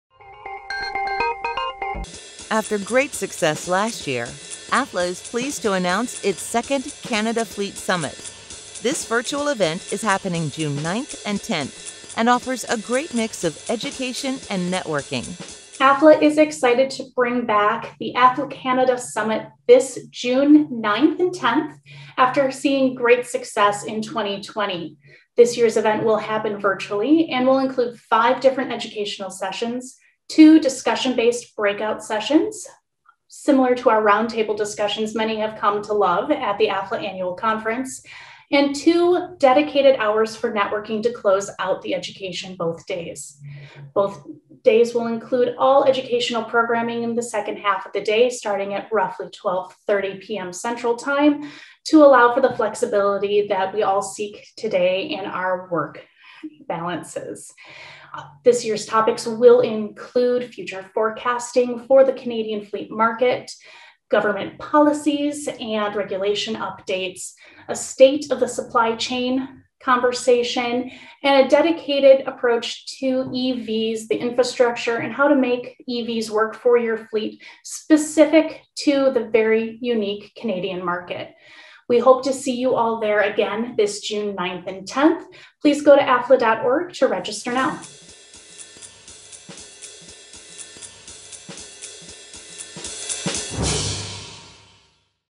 Attend the AFLA Canada Fleet Summit Online | ELIZABETH SCHLICHT | Fleet Management Weekly
After great success last year, AFLA is pleased to announce its second Canada Fleet Summit. This virtual event is happening June 9th and 10th and offers a great mix of education and networking.

ELIZABETH SCHLICHT
Executive Director
AFLA